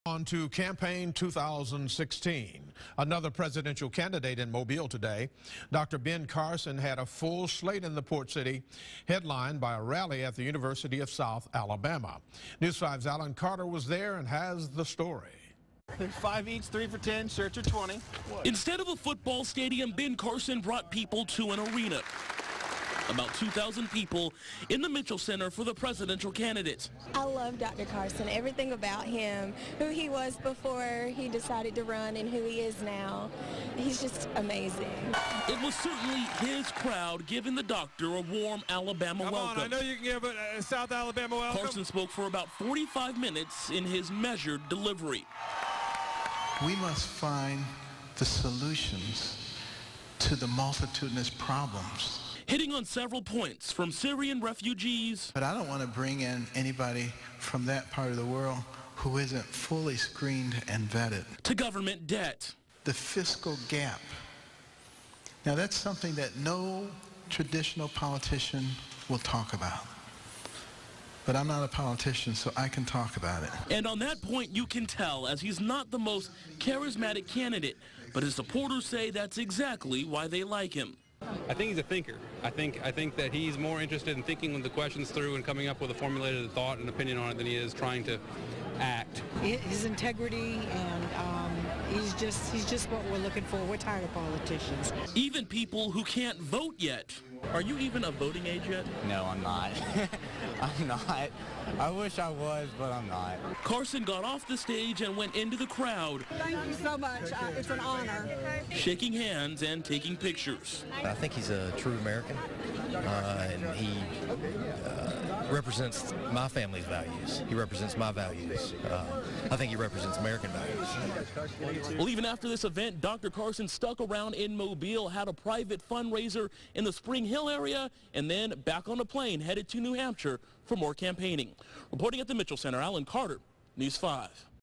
Ben Draws Hundreds at USA Rally
Carson spent a day in Mobile campaigning and then holding a fundraiser afterward.